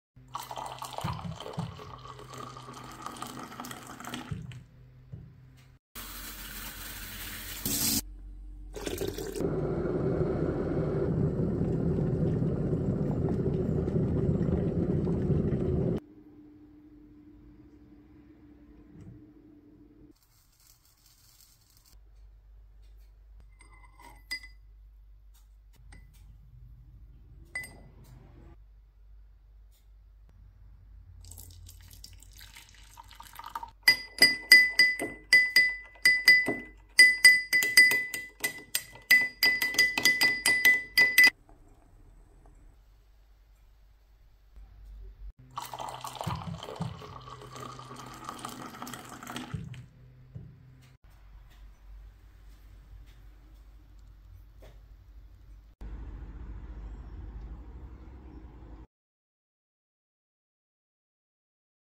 How To Make Perfect Nescafe Coffee In 5 Minutes Without Coffee Maker
How to make best nescafe coffee

Ingredients
Sugar
Nescafe coffee Powder

Take Milk and Water And Boil it
Take nescafe coffee powder and Sugar in a glass
Add some Boiled Milk
Whisked the Mixture By a Spoon for Minimum 2-3 minutes
Pour the Remaining hot milk mixture into Coffee Mixture And Mix well

Coffee is ready to drink

nescafe coffee#mallu